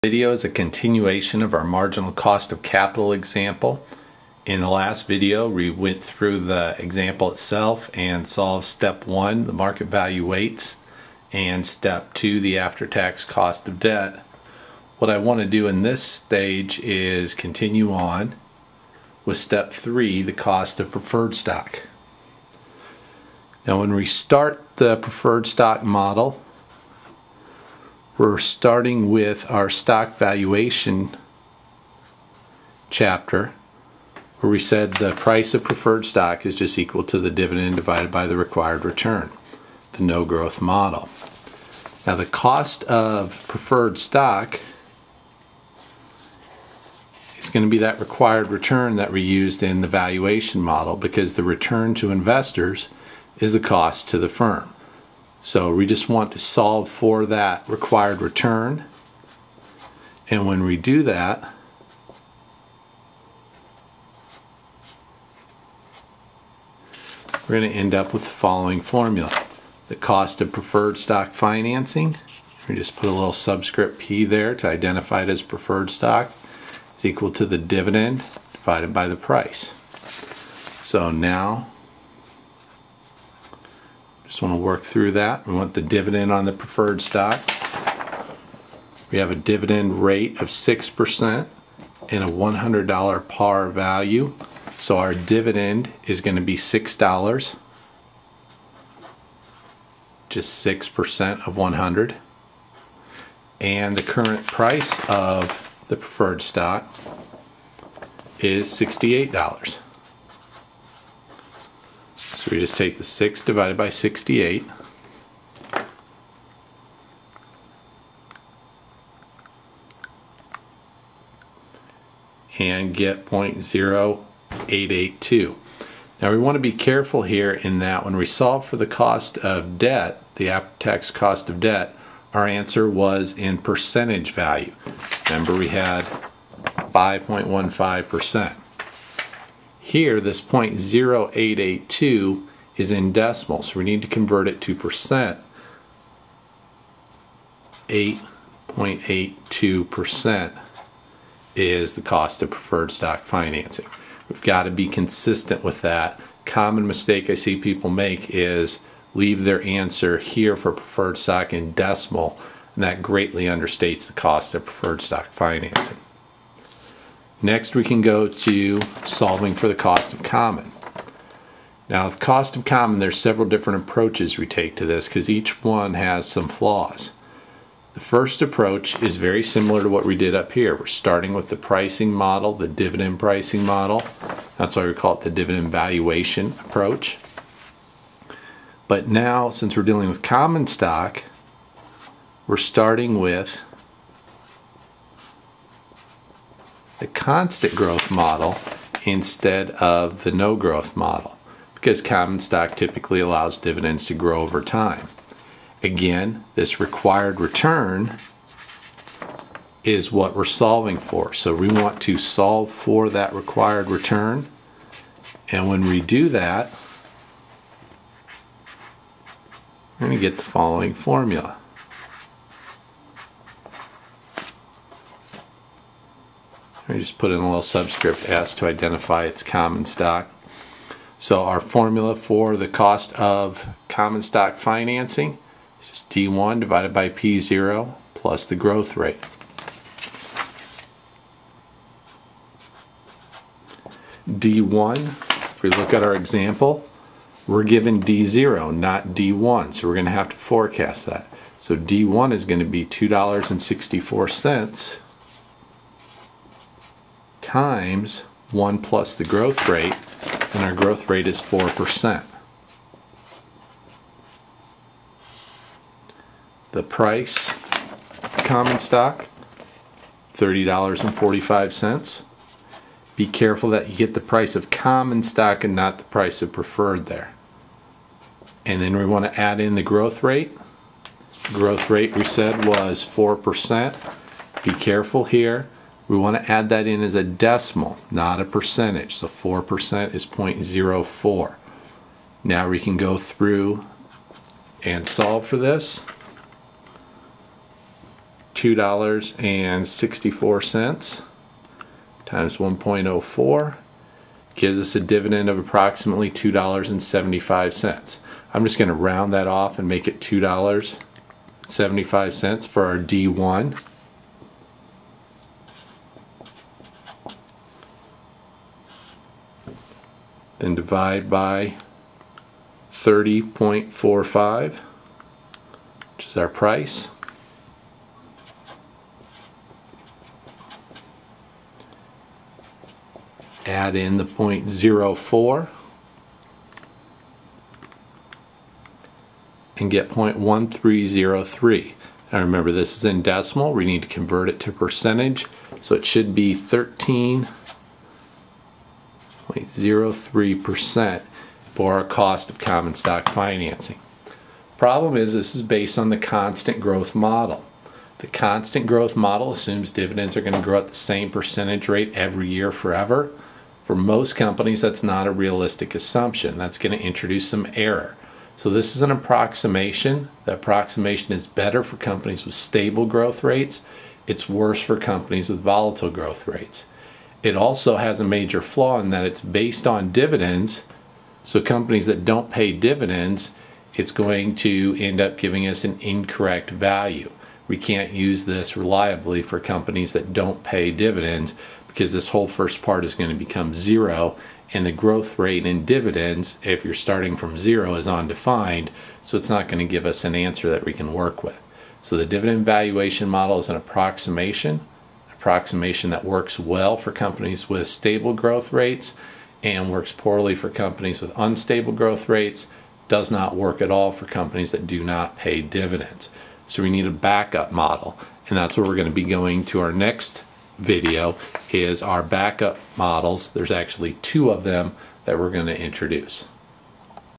Marginal Cost of Capital Part Two of Four
Marginal Cost of Capital Example in Four Parts.  Part two goes calculates the cost of preferred stock and uses starts the cost of common with the dividend valuation model.